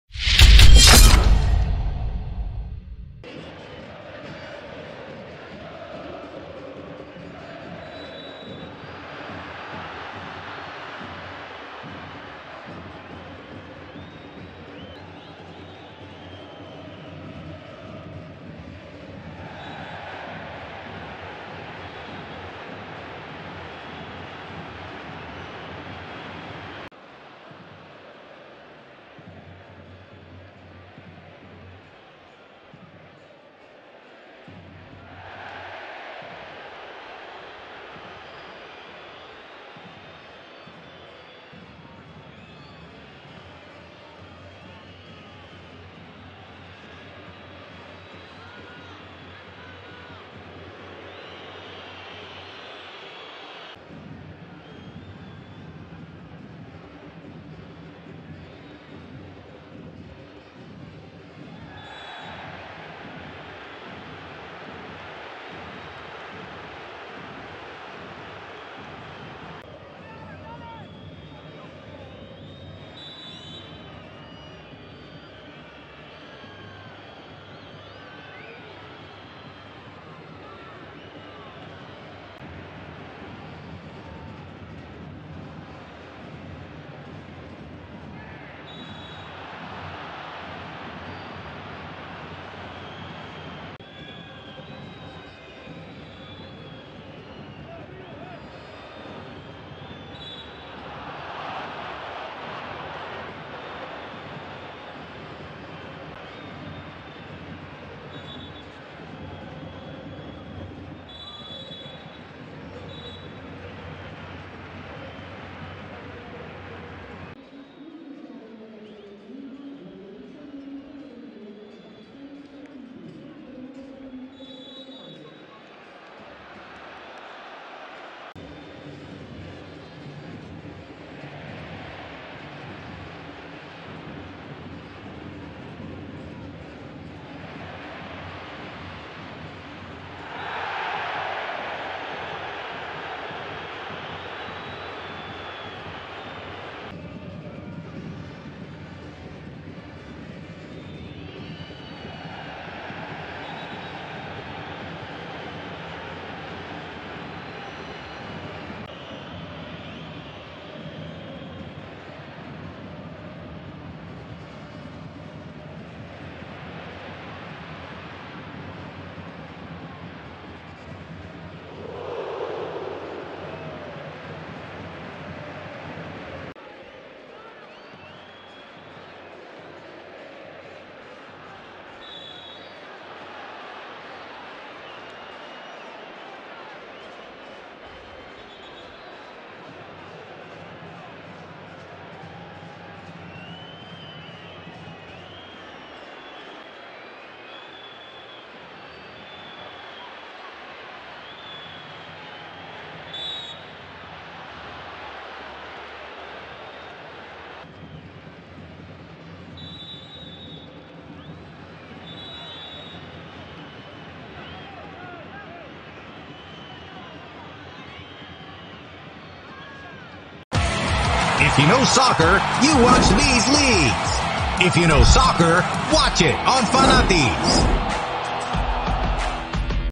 Huracán 0-0 San Lorenzo: Game Highlights | CopaSurFinanzas 2024
0-0 draw between Huracan and San Lorenzo at Parque Patricios

Huracán and San Lorenzo played to a 0-0 draw. This short recap highlights the key moments of the match - don't miss the excitement of every goal! ⚽🔥